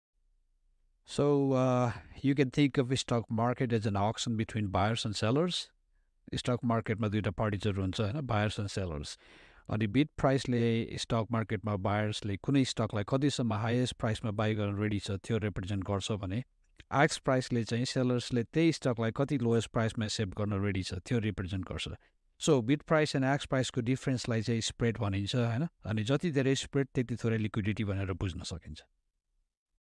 Beginner Trader: Bid Vs Ask Price
It's important to know the Concept and difference between the Bid and Ask price.